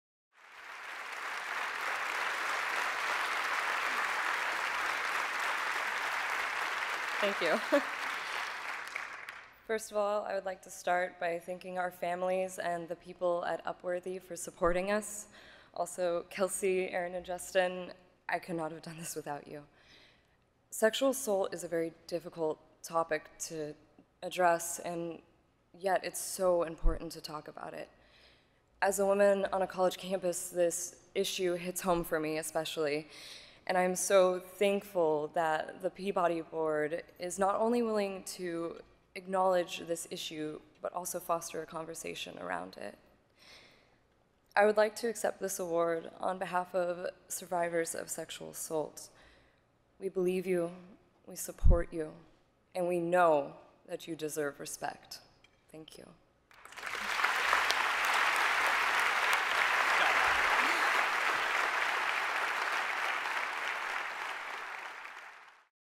Samantha Stendal - A Needed Response - 2013 Peabody Award Acceptance Speech
Winner 2013 | Samantha Stendal, Aaron Blanton

The first viral video to be honored with a Peabody Award, A Needed Response grew out of two University of Oregon students’ outrage upon watching news coverage of the Steubenville, Ohio, rape sentencing that bemoaned the perpetrators’ loss of their promising athletic careers while ignoring the 16-year-old girl they had drugged and assaulted. Samantha Stendal and Aaron Blanton conceived, staged and shot a 26-second video that emphatically rejects the idea that rape is ever excusable. Short, simple and spot-on, it’s an ingenious public-service message targeting college age viewers. Stendal and Blanton shared their video on Upworthy’s Facebook page. Within 48 hours, the video surpassed 1 million views, an astonishing figure that doubled, tripled and continued to grow as more and more people shared it and debated its take on masculinity and morality. For making a unforgettable, undeniable statement about rape culture and sharing it with the world via social media, A Needed Response receives a Peabody Award.